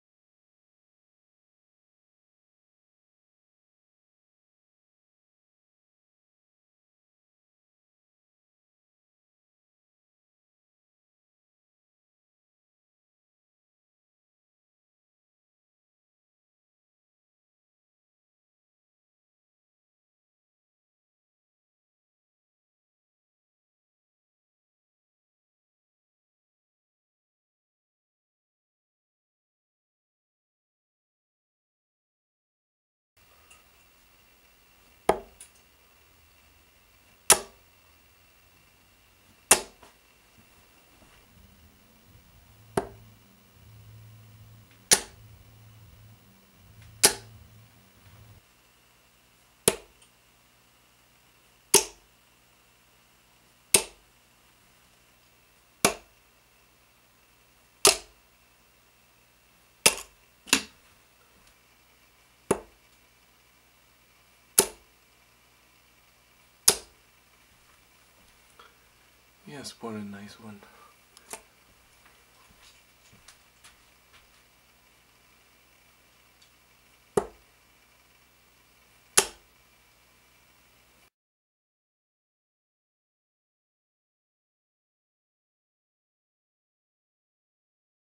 2014 Unicorn Jamie Caven Darts - 180 HD
A little picture review and a few 180´s with:
2014 Unicorn Jamie Caven darts in 23g with 90% tungsten.
Jamie´s new darts have a scalloped part at the back of the barrel where you can place your thumb and pointer finger at. This part has a micro grooved grip. The rest of the barrel has a shark grip. On a scale to 5 the scalloped part has a level 4, the shark grip a level 5 grip. These are really nice darts, well ballanced and they fly very nice.